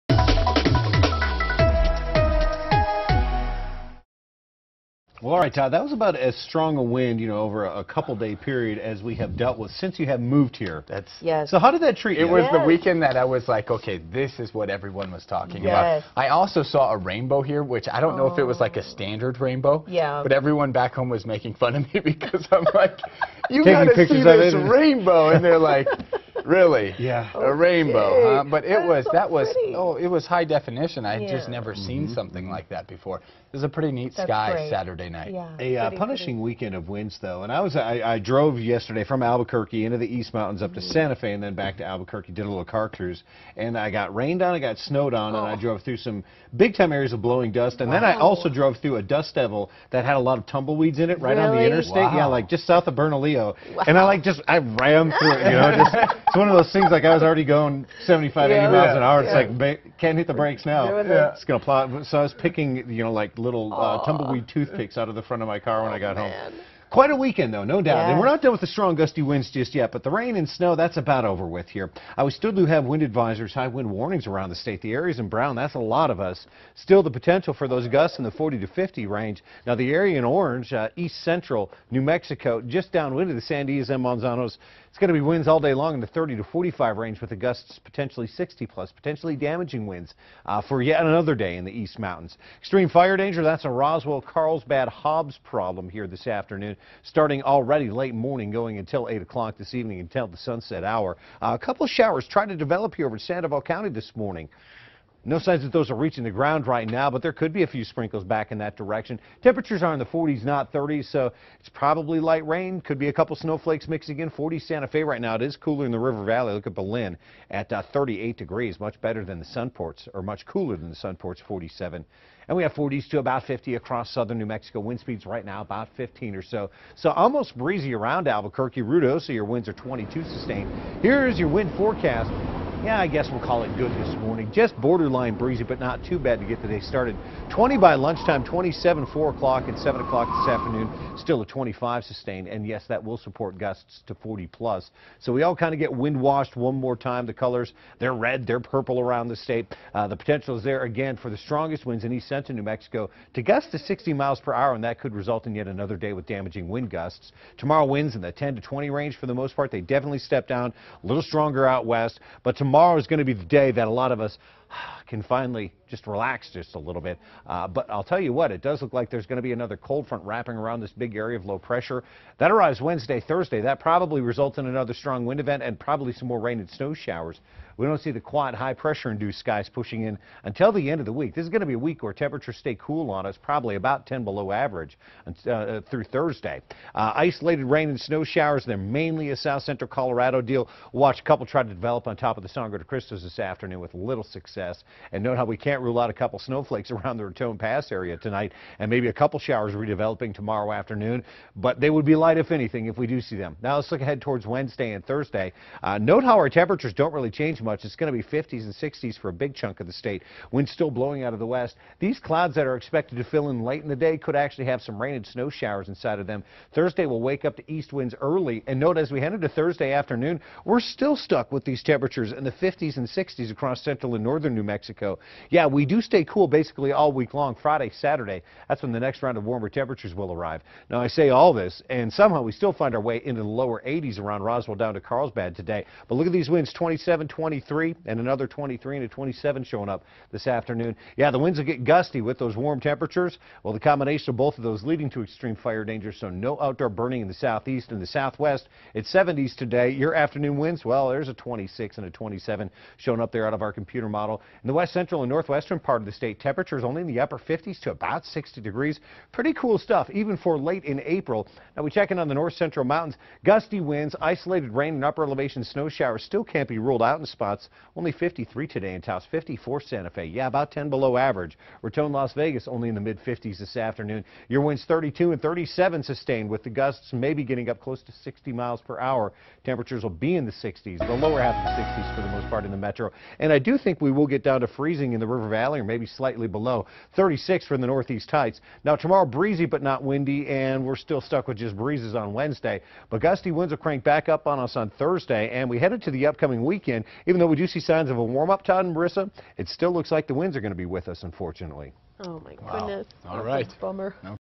Eric Weather Forecast for Monday April 28th
Windy today with temperatures staying in the mid 60s until friday